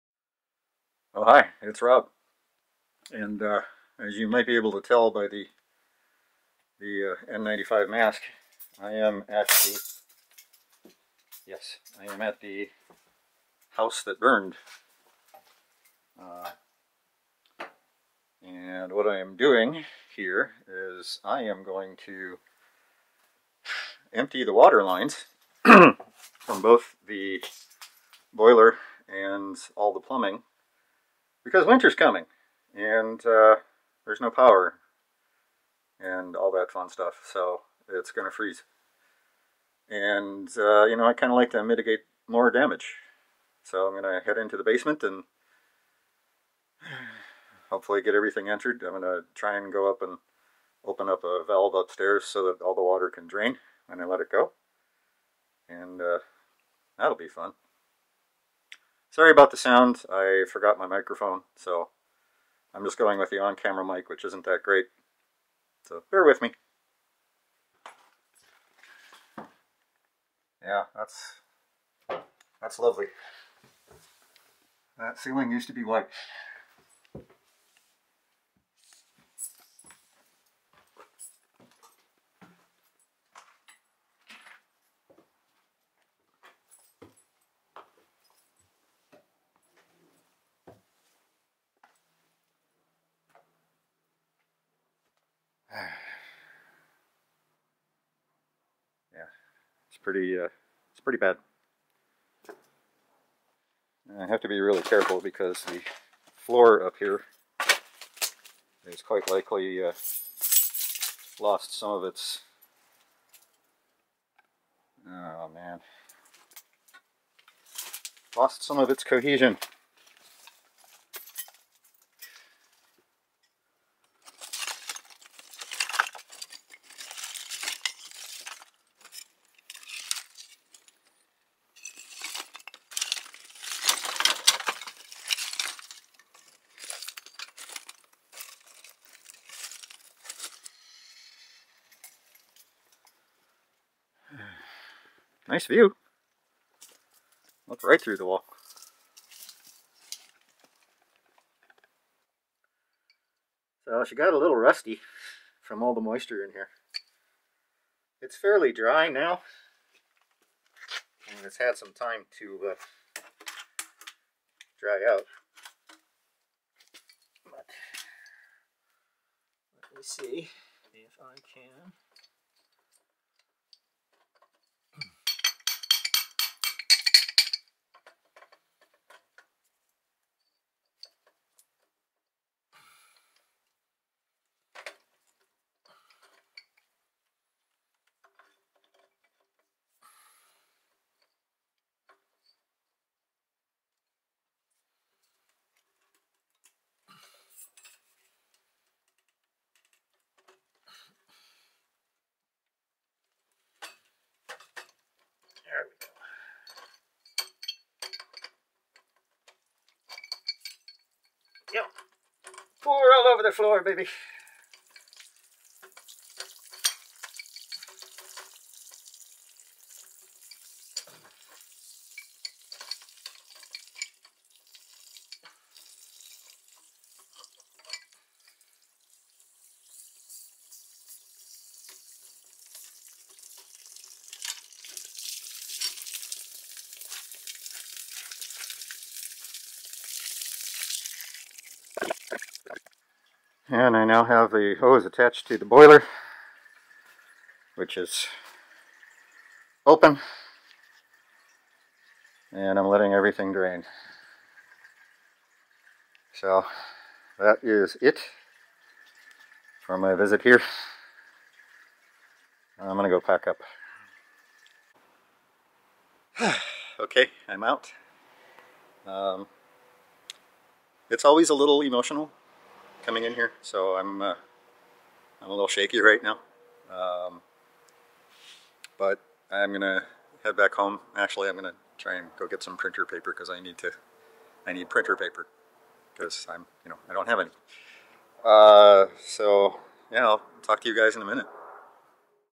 Draining the water in the burned house
Winter is coming, and there is no heat in the house. To help mitigate any freeze damage, I had to go in and drain the plumbing and the boiler.